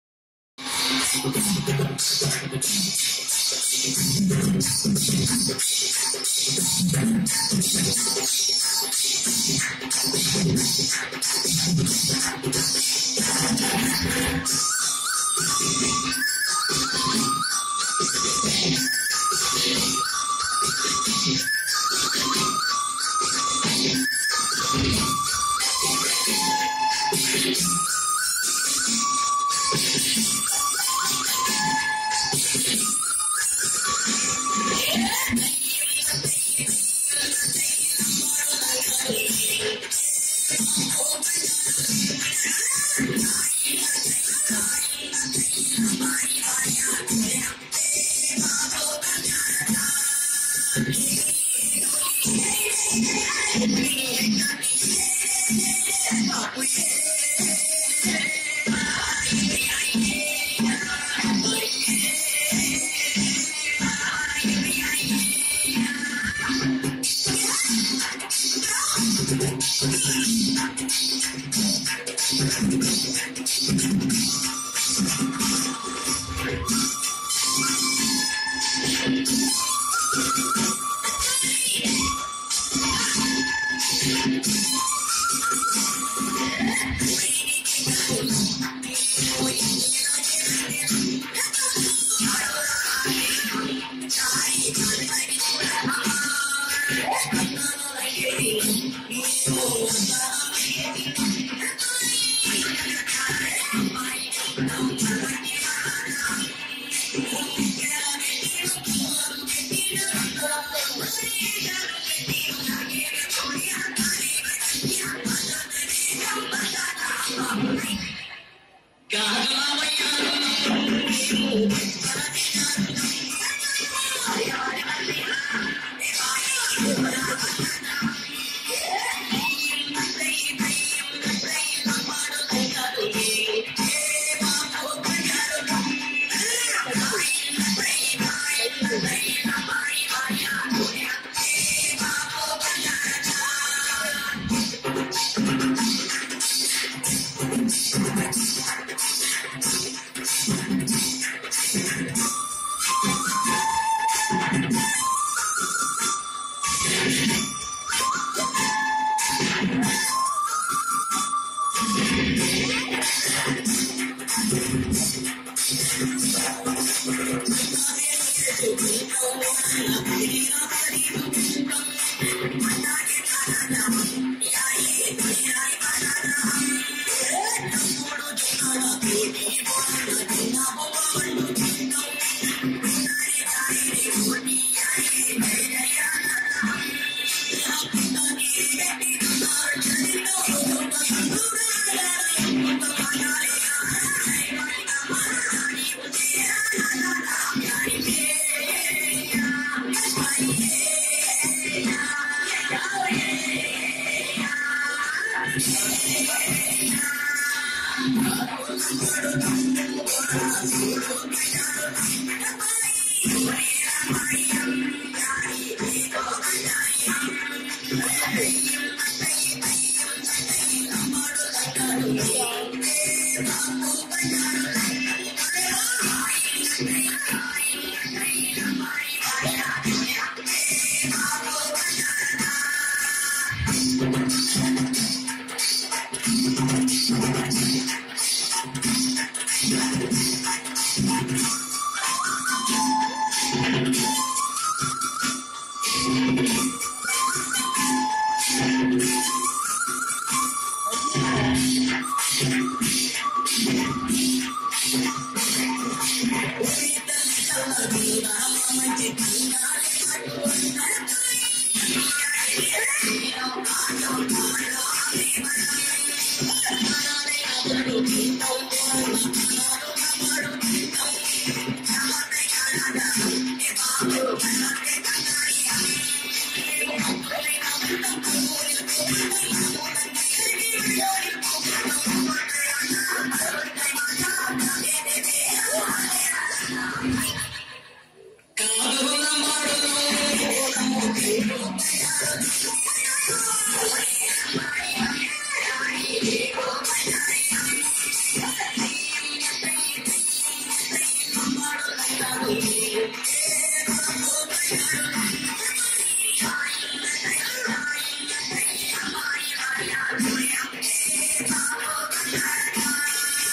JKKN school annual day 2018
3rd 4th and 5th class LAMBADI dance